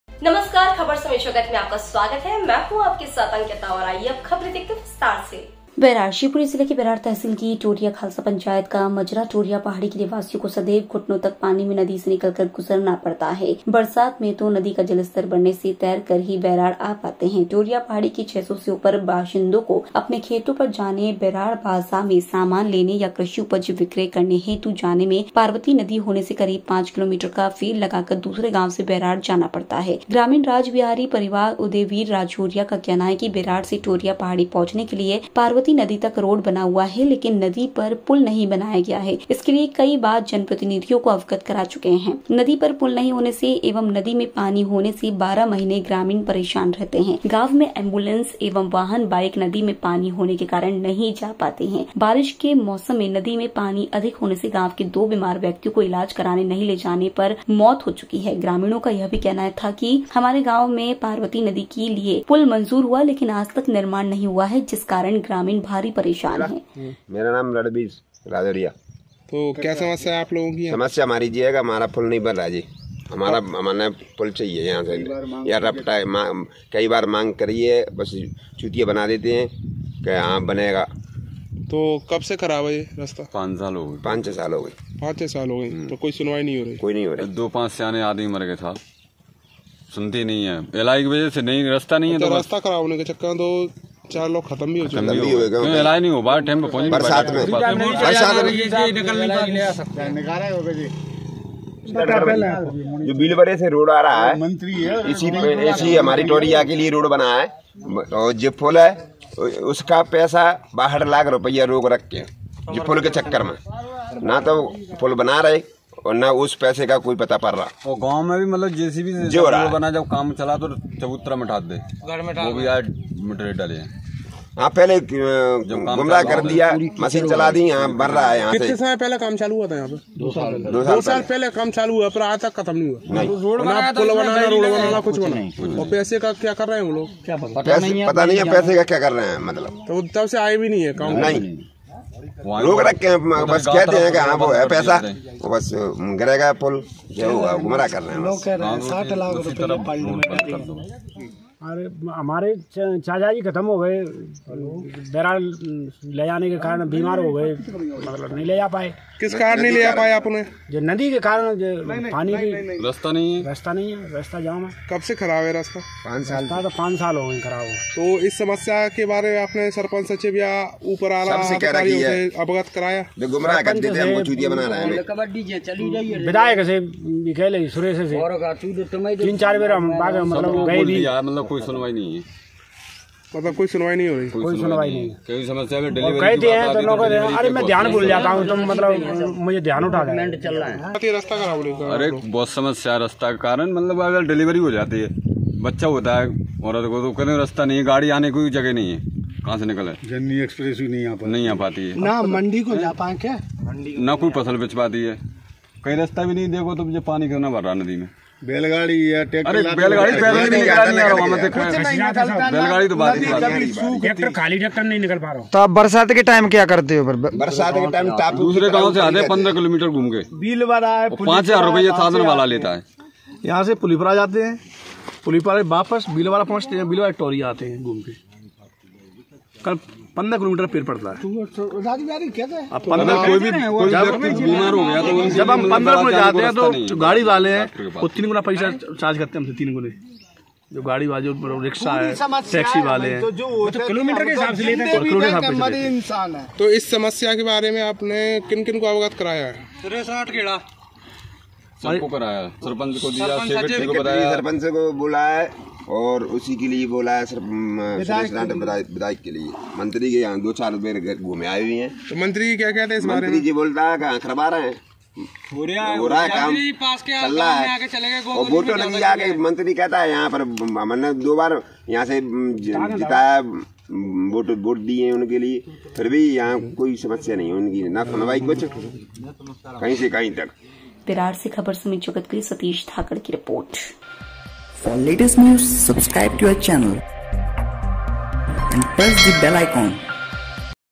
बैराड़:- टोरिया पहाड़ी के निवासी नदी पर पुल नहीं बनने से परेशान
सतीश धाकड़ की रिपोर्ट